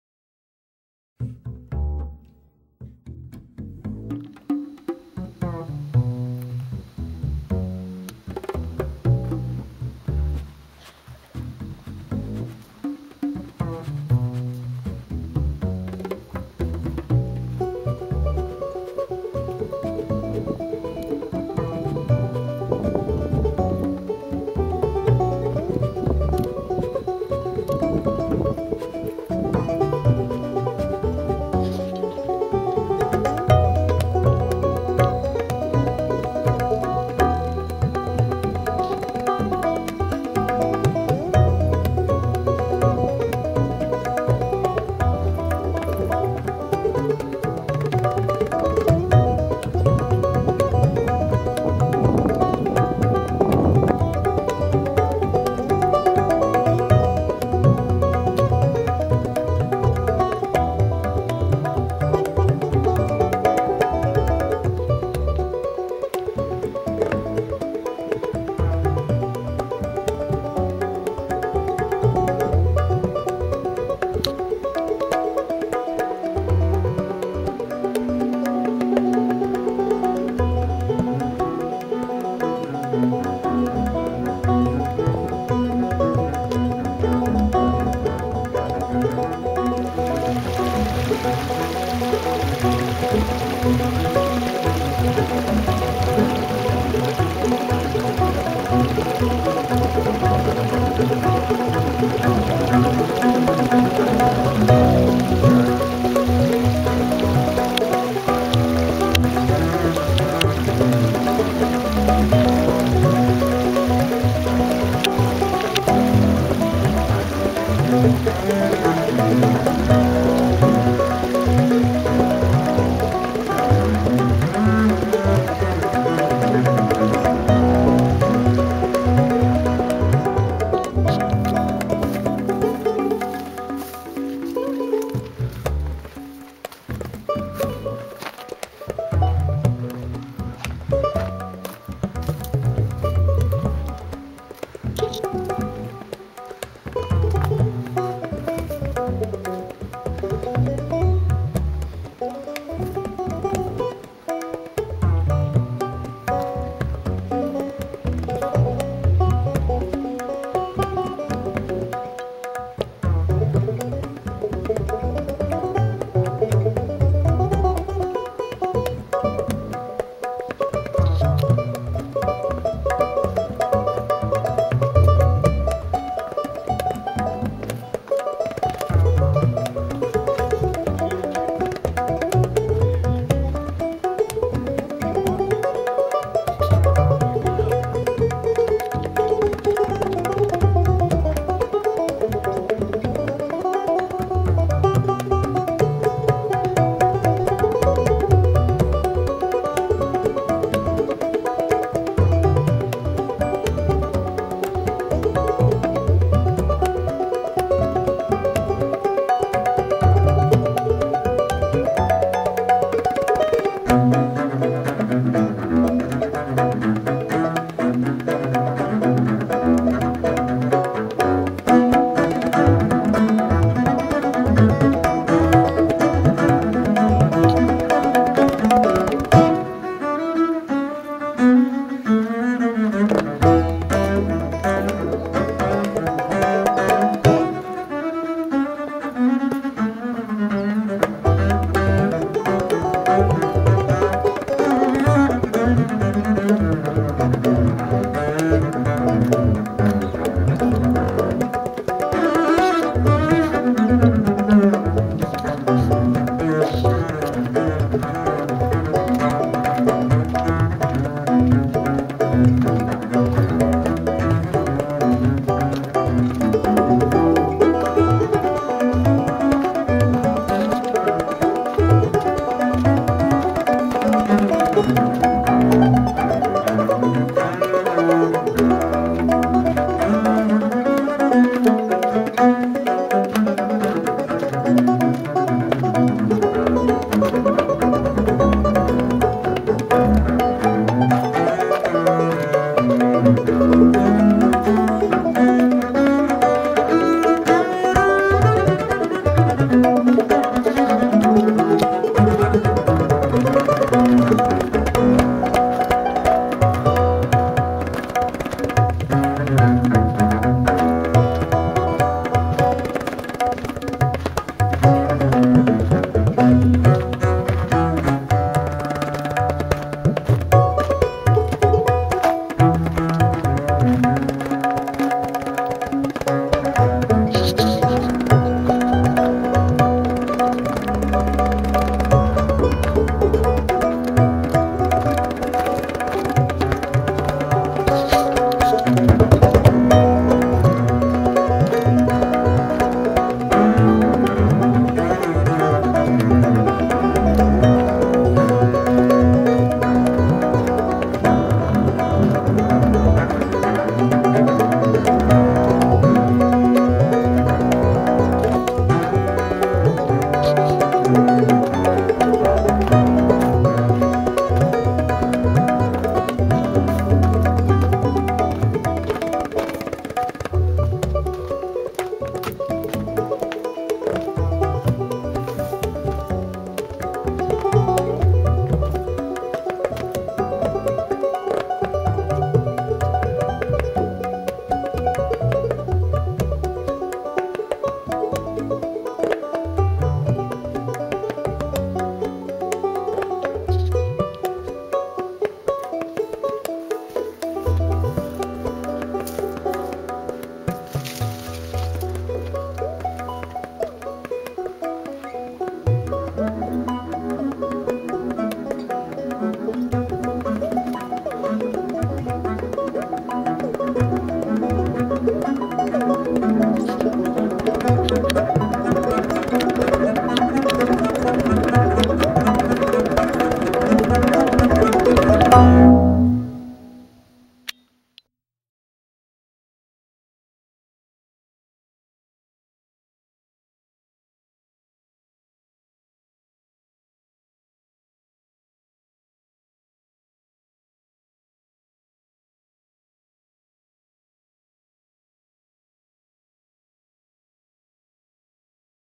BIG BEND NATIONAL PARK Santa Elena Canyon Trail
A hike into Santa Elena Canyon in Big Bend National Park, Texas. The Rio Grande and Mexico are on the left, Texas is on the right, and Terlingua Creek comes in from the north. Wednesday, November 4, 2009.